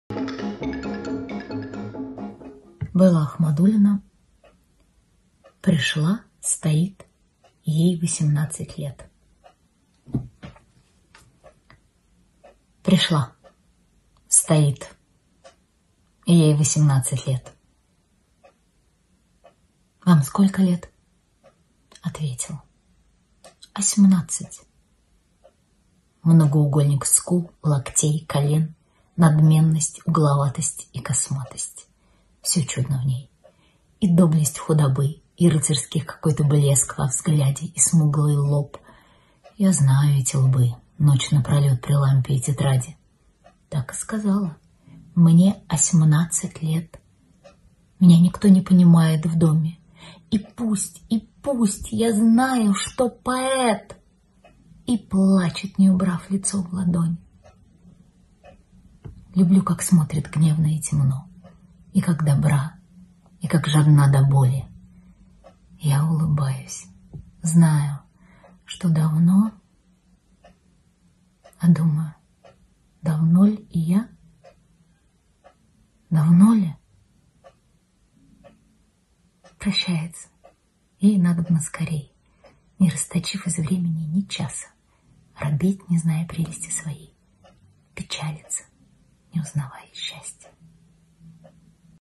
Белла Ахмадулина – «Пришла. Стоит. Ей восемнадцать лет…». Читает Виктория Толстоганова
15 апреля, в день рождения «Современника», на сайте театра состоялась премьера онлайн-проекта «Снова оттепель. Поэзия времени». В нем приняли участие 52 человека, в большинстве своем — артисты «Современника». Они записали 112 видео, где читают стихи и прозу оттепели — легендарного и невероятно значимого для истории и культуры нашей страны времени. Каждый день в 10.00 публикуем что-то одно из нашей прекрасной коллекции.

Сегодня Виктория Толстоганова с лирикой Беллы Ахмадулиной.

А также каждый день в 19.00 - премьера новых серий проекта "Доктор Чехов"